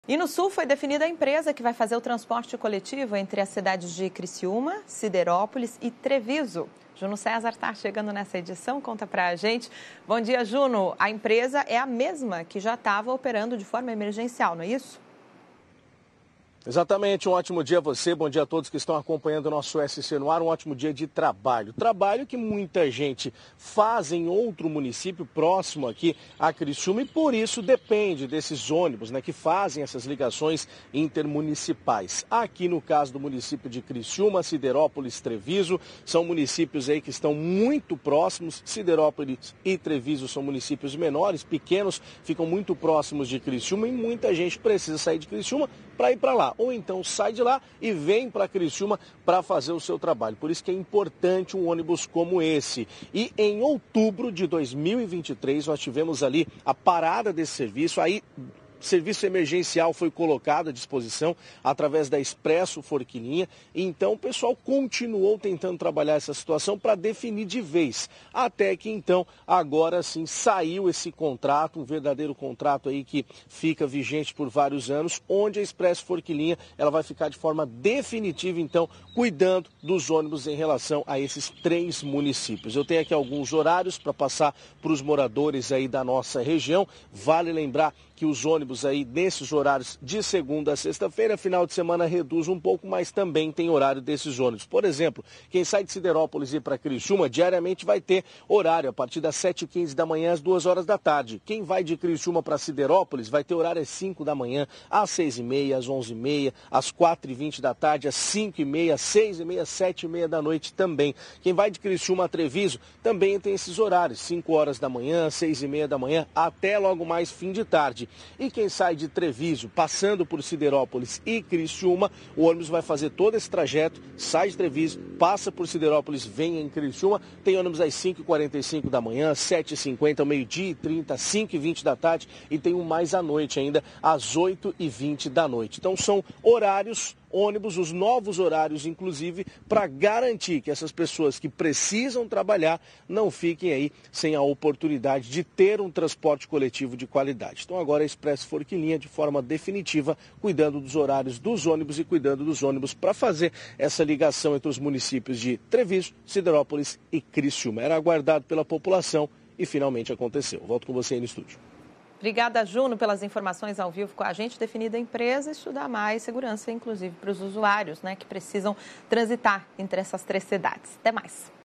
Nova empresa ampliará horários de ônibus entre Criciúma, Siderópolis e Treviso
Após a desistência da empresa anteriormente responsável, o Ministério Público autorizou a contratação emergencial de uma nova empresa para operar as linhas de ônibus entre Criciúma, Siderópolis e Treviso. A Secretaria de Estado da Infraestrutura e Mobilidade concluiu com sucesso o Processo de Seleção Simplificada para essa prestação de serviço de transporte intermunicipal de passageiros. A mudança trará a ampliação dos horários disponíveis para os usuários dessas rotas.